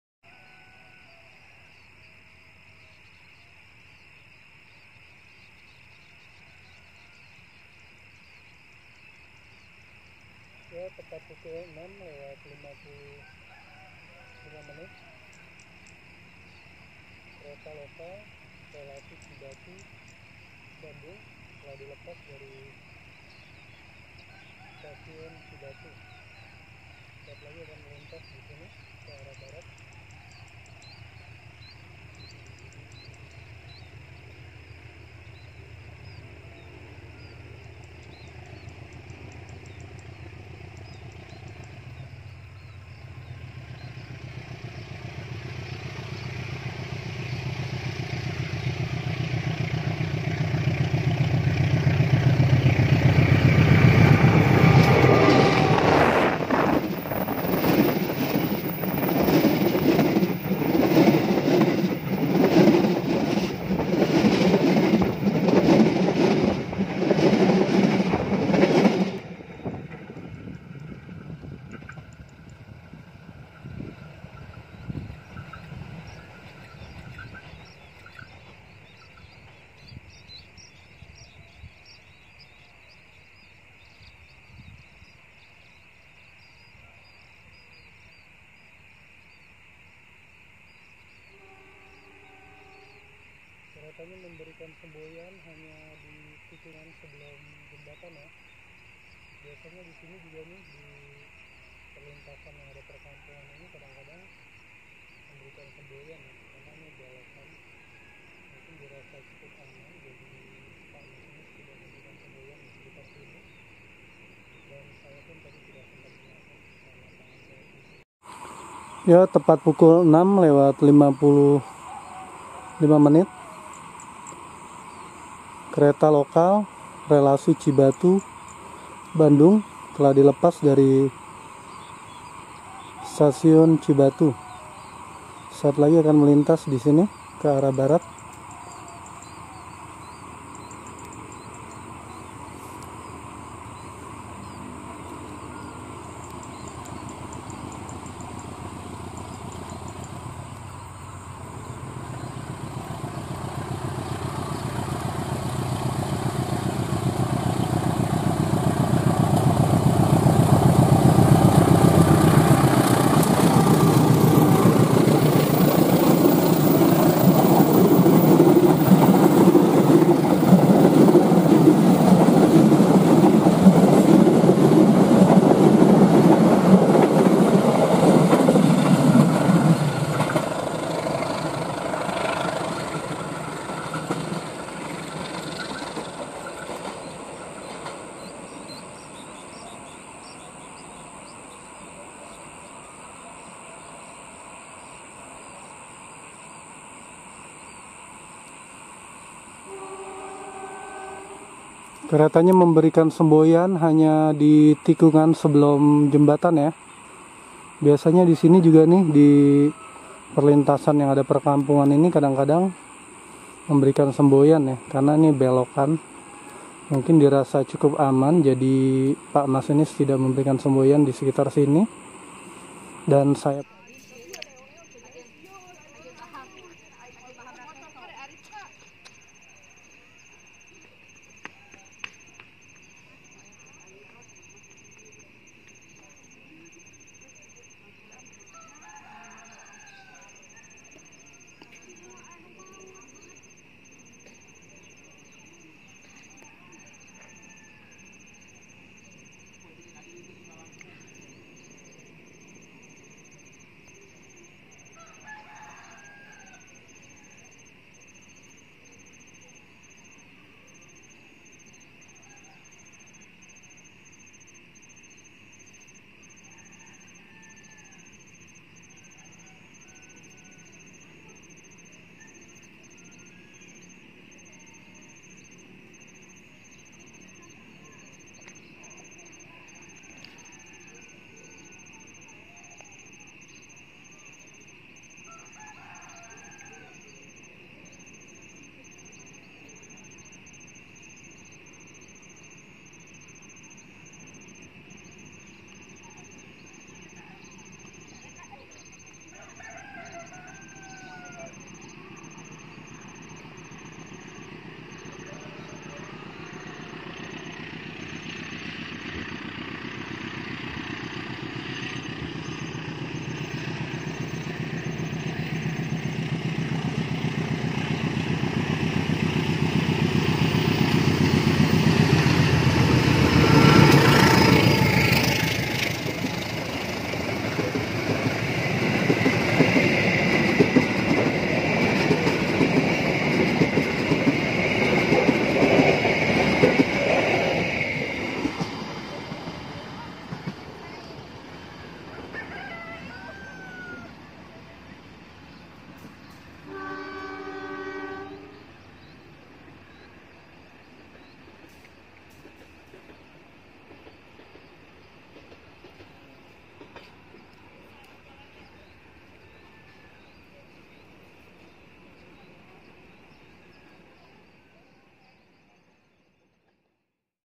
Gagahnya Kereta Lokal Cibatu Bandung Melintas Di Leuwigoong Garut Menuju Bandung
kereta lokal Cibatu Bandung biasa berangkat dari stasiun Cibatu pada pukul 06.55 dengan harga tiket sangat ekonomis Rp.7000